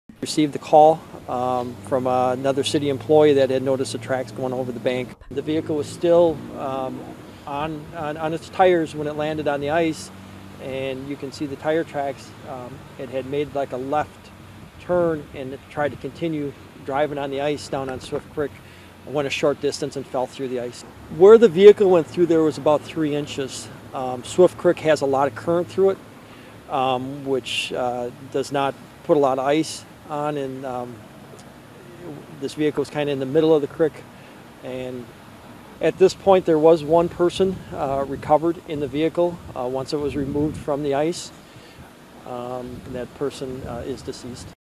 Man Dies As Car Falls Through Ice In Wisconsin
A man has been killed when his car veered into the Mississippi River ice and fell through a hole.

The man was pulled from the vehicle but died, officials said.

Sergeant Randy Rank in La Crosse, Wisconsin, said police were alerted by a caller who had noticed tracks going over a bank.

"The vehicle was still on its tyres when it landed on the ice," he said.

"You can see the tyre tracks, it had made a left turn and tried to continue driving on the ice on Swift Creek, went a short distance and fell through the ice."

The ice was not thick enough to sustain the weight of the car, which was towed out of the water after a diver attached cables to it.

The driver was believed to be the only person in the vehicle.